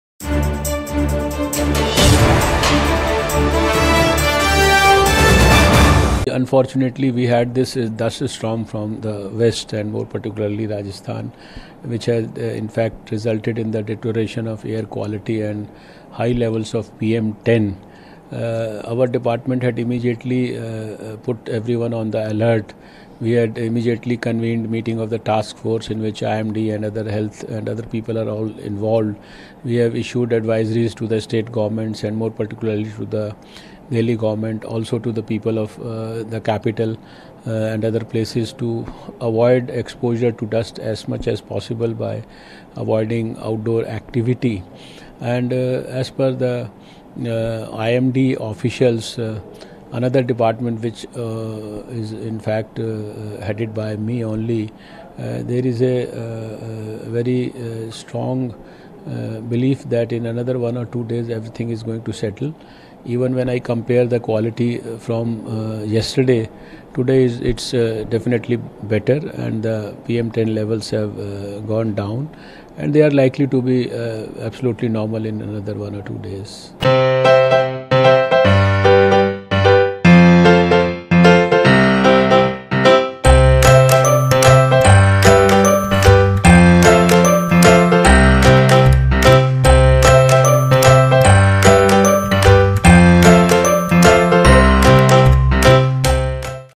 धूल के गुबार ले 1-2 दिनों में मिलेगी राहत- पर्यावरण मंत्री
Delhi’s air quality should be normal in 1 2 days Environment Minister